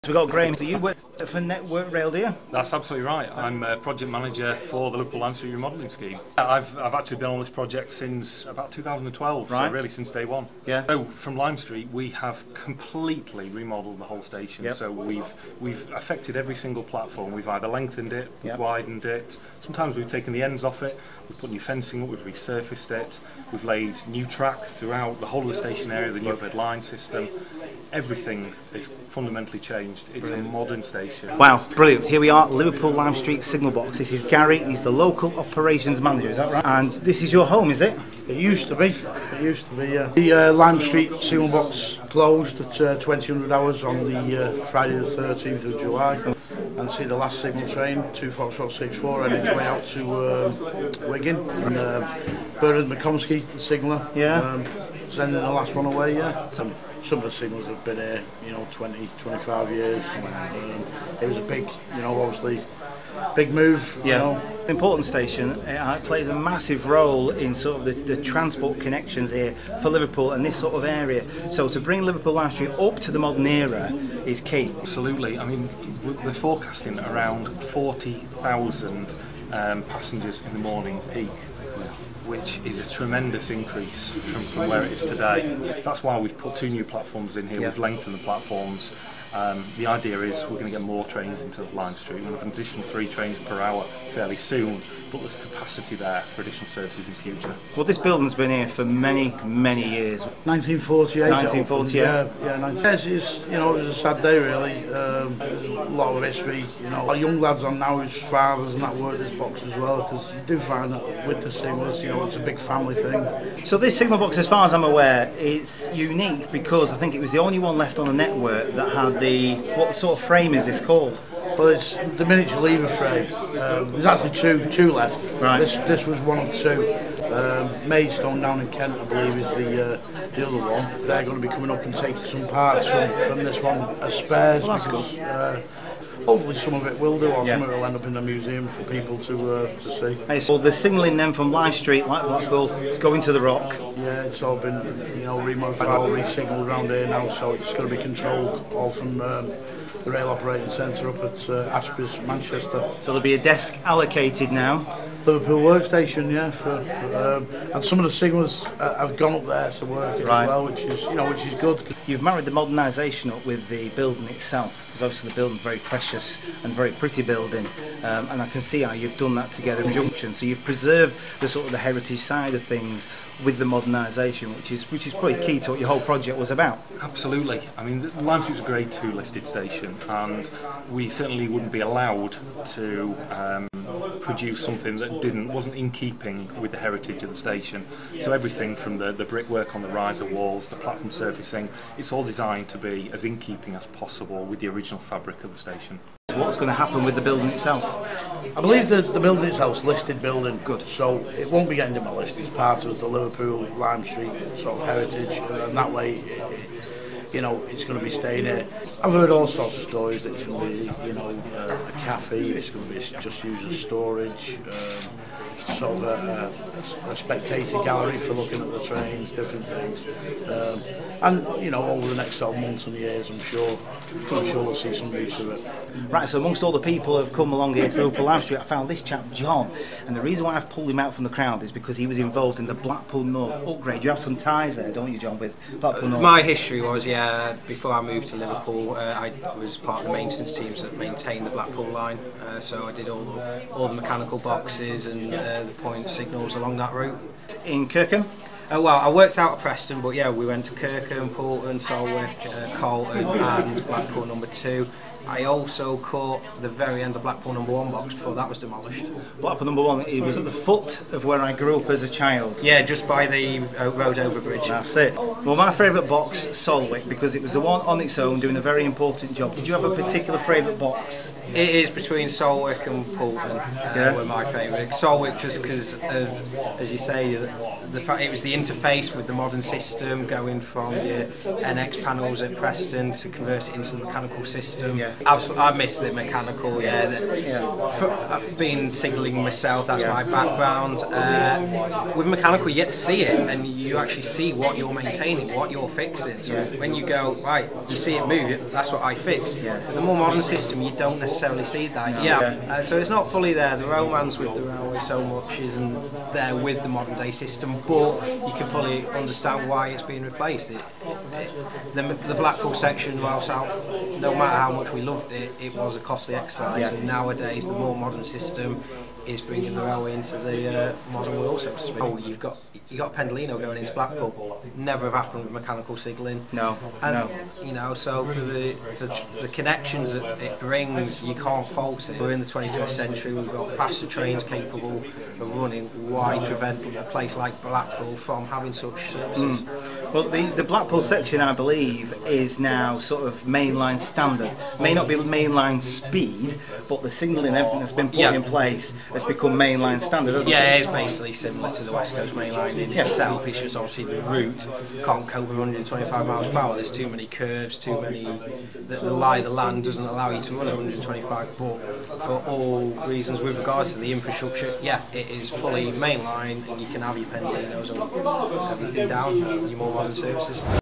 Explore Liverpool Lime Street Signal Box
An invite by Network Rail to the now disused signal box can not be missed.  At 20:00hrs on 13th July Liverpool Lime Street Signal Box gave out it's last instruction. So what will they do with this Grade 2 listed building now the whole upgrade to Lime St Station included all signalling to be moved to Manchester Railway Control.

Work on the listed station, new platforms, improved drainage and much more. Today's vlog  is a great opportunity to speak to some of the main players involved on the upgrade and signal box.

Nodrog ©️